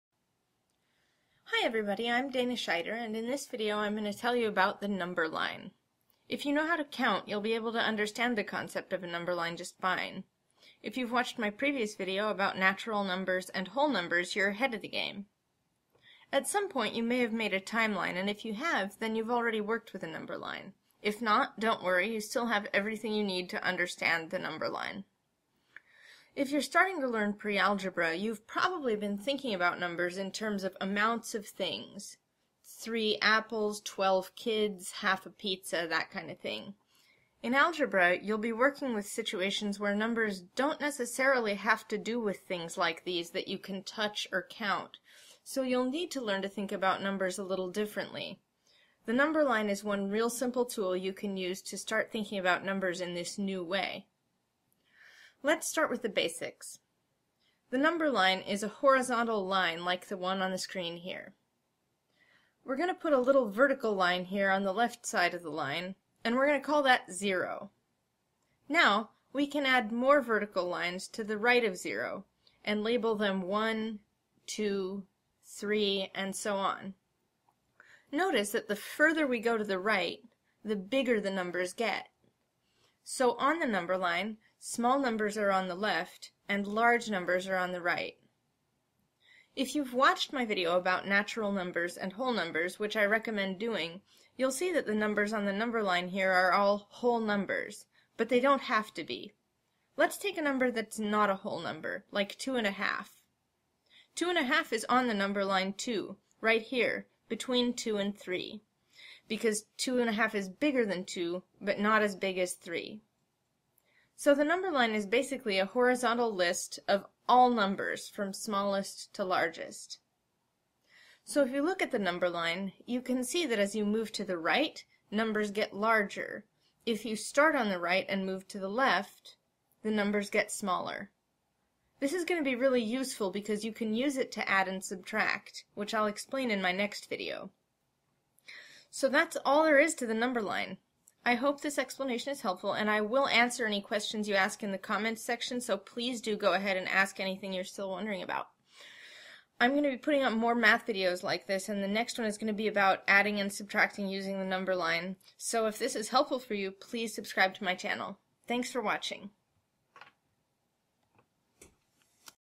Pre-Algebra Made Easy: The Number Line (Lesson 2)
This video explains the concept of the number line. If you can count, you can understand the number line.

I answer all questions posted in the comments section and also take requests for new videos.